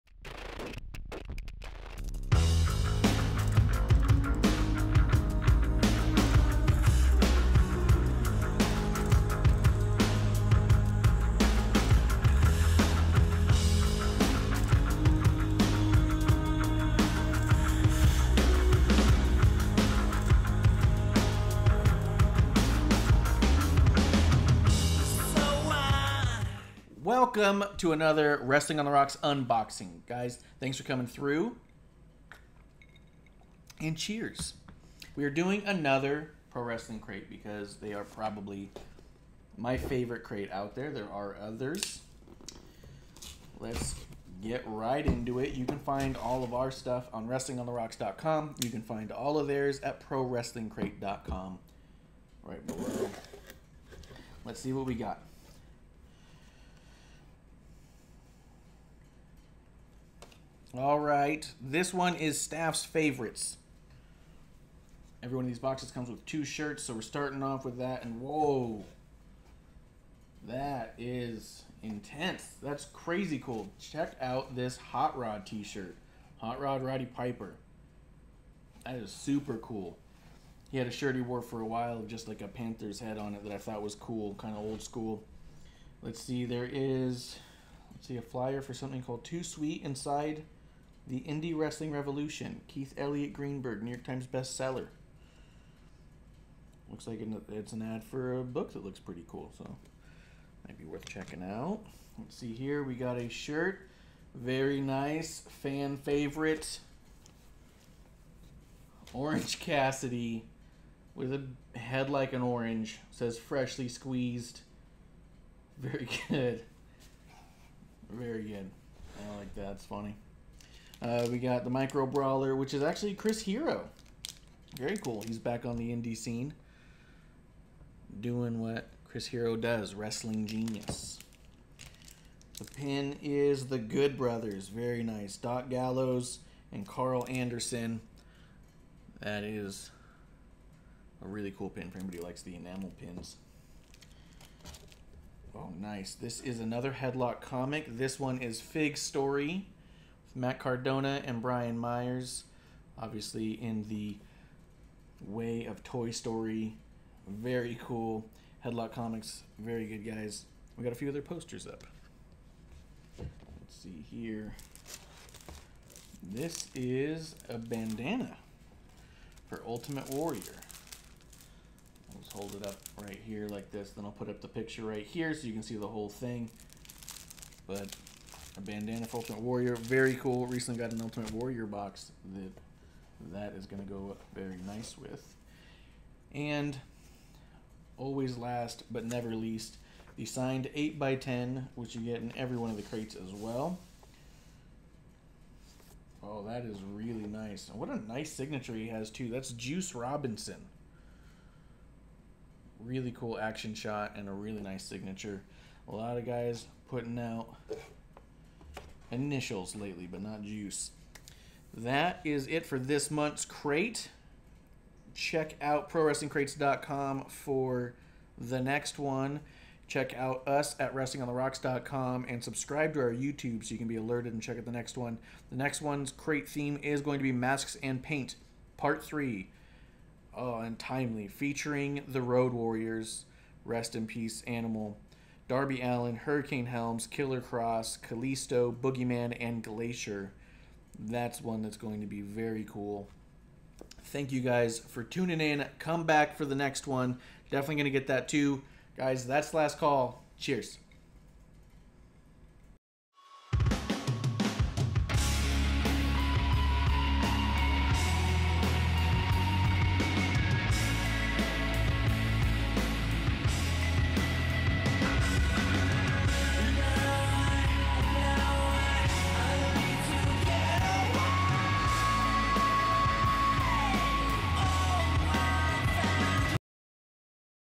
ProWrestling Crate September 2020 Unboxing - Staff Favorites theme
Unboxing of this month's Prowrestling Crate! Two shirts, Enamel Pin, Micro Brawler, DVD, signed 8x10 and more! Grab a drink and let's take a look! Cheers!

Wrestlingontherocks.com

ProwrestlingCrate.com